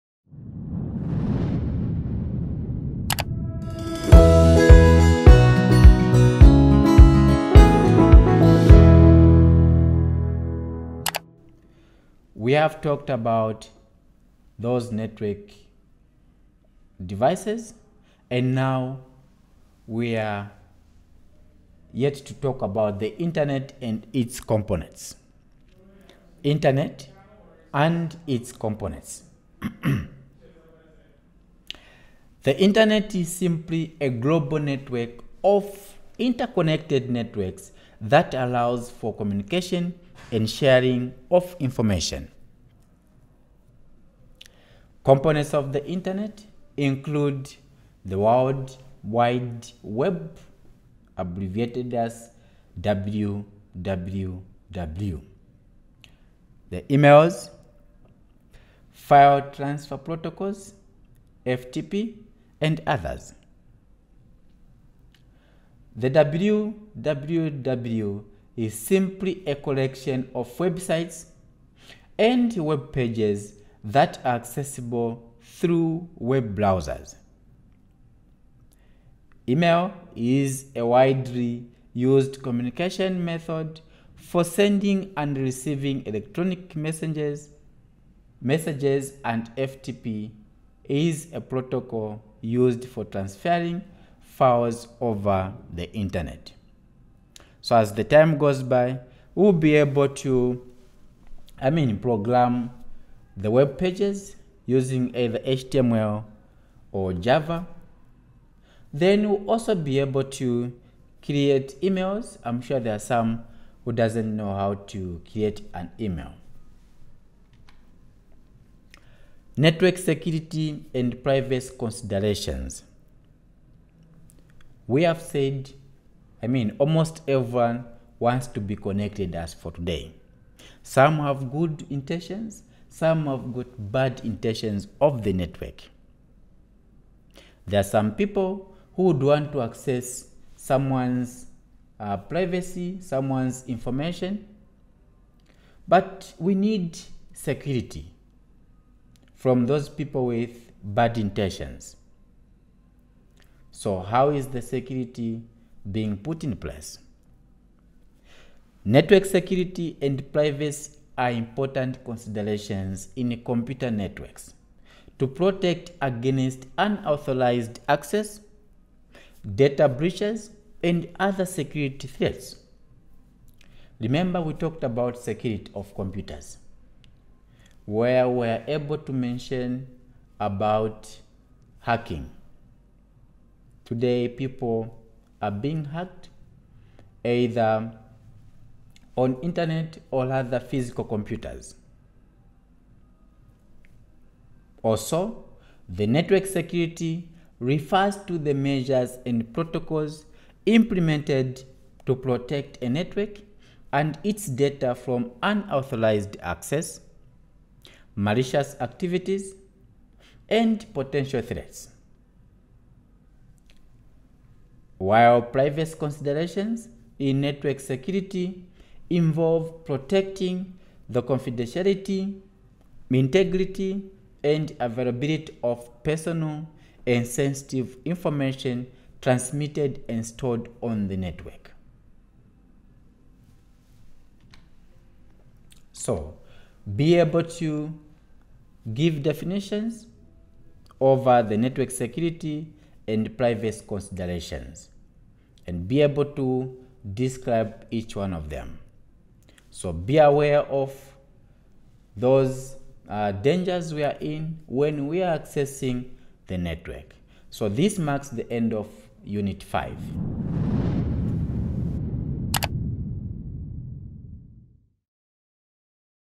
The Internet and Its Components
This video discusses the internet and its components.

The Internet is a global network of interconnected networks that allows for communication and sharing of information. Components of the Internet include the World Wide Web (WWW), email, file transfer protocols (FTP), and others. The WWW is a collection of websites and web pages that are accessible through web browsers. Email is a widely used communication method for sending and receiving electronic messages, and FTP is a protocol used for transferring files over the Internet.